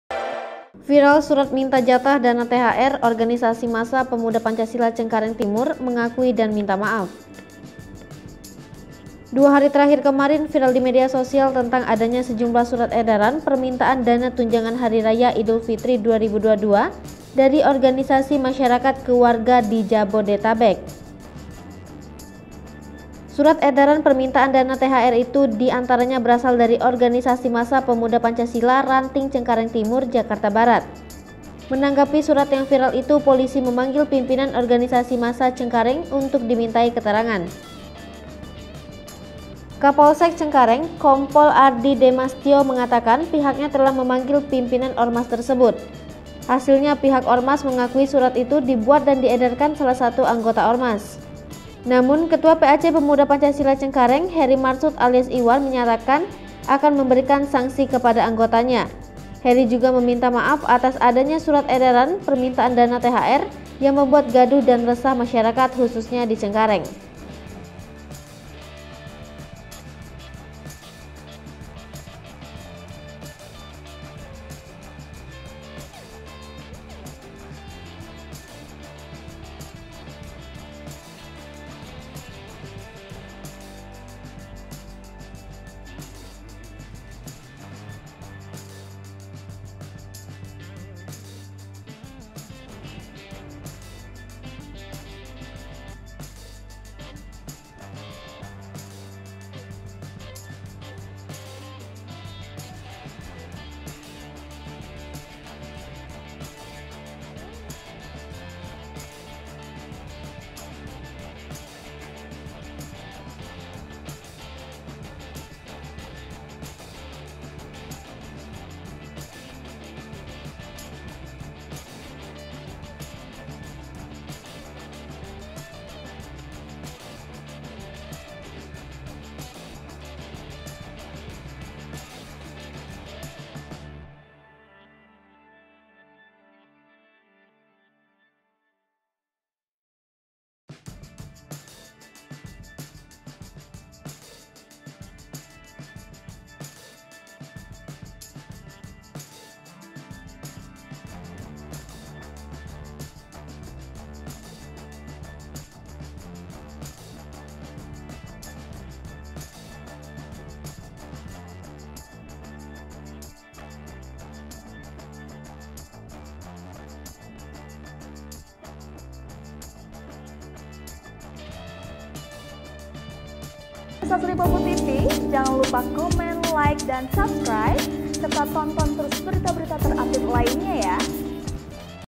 Viral Surat Minta Jatah Dana THR, Ormas PP Cengkareng Timur Mengakui dan Minta Maaf
Surat edaran permintaan dana THR itu di antaranya berasal dari ormas Pemuda Pancasila (PP) Ranting Cengkareng Timur, Jakarta Barat.

Menanggapi surat yang viral itu, polisi memanggil pimpinan Ormas Cengkareng untuk dimintai keterangan.

Kapolsek Cengkareng, Kompol Ardhie Demastyo mengatakan pihaknya telah memanggil pimpinan ormas tersebut.

Hasilnya, pihak ormas mengakui surat itu dibuat dan diedarkan salah satu anggota ormas.

Lihat informasi terkini bersama kami, di:

Palembang.tribunnews.com

Sumber Video: Jakarta.tribunnews
Editor Video: Sarah Hanifah